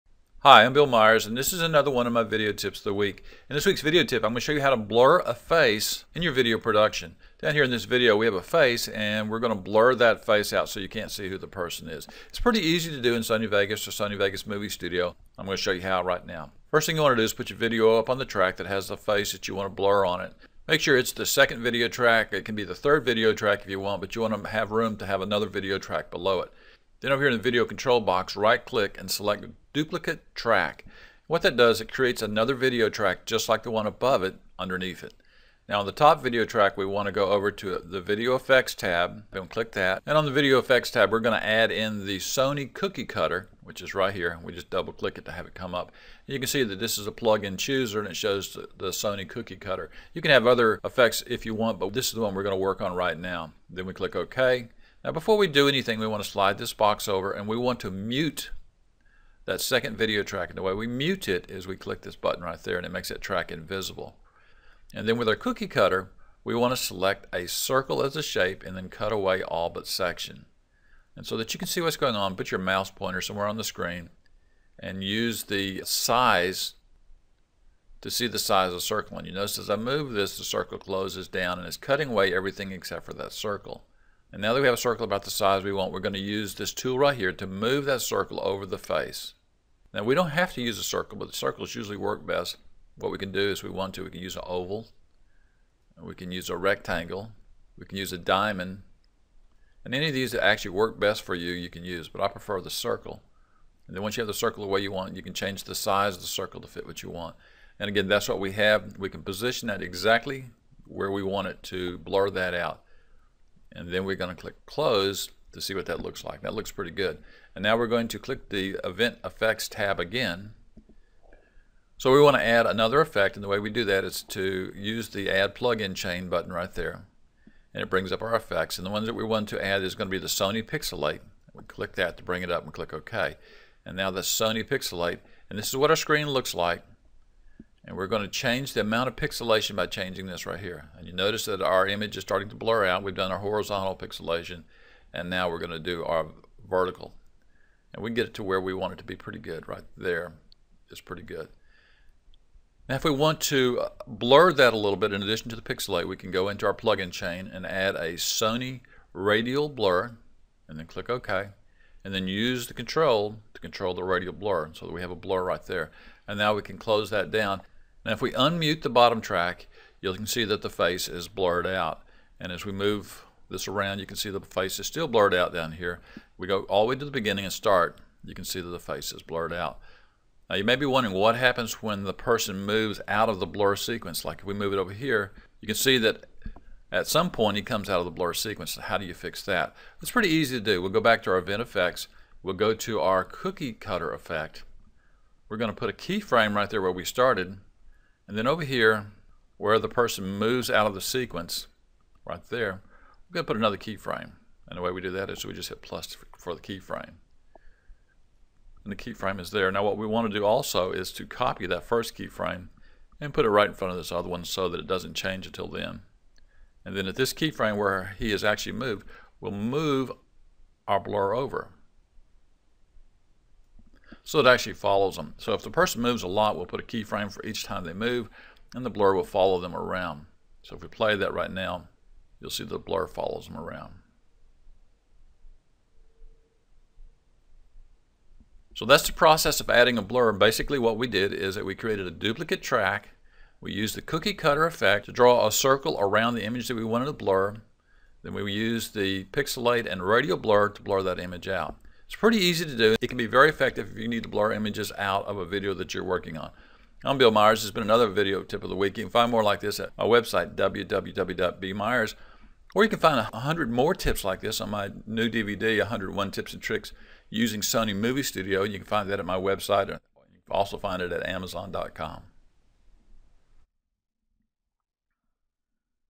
Blur out or pixelate faces or objects with Sony Movie Studio
Blur out or pixelate faces in your videos with Sony Movie Studio. Easy to do once you see this video.